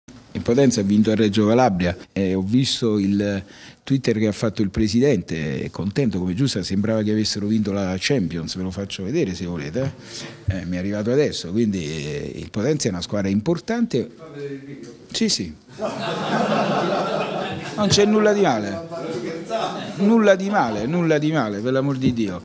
Avellino, "Tirate fuori il video": atto secondo tra le risate
Siparietto Capuano: "Ho visto il video del presidente Caiata, sembrava avesse vinto la Champions"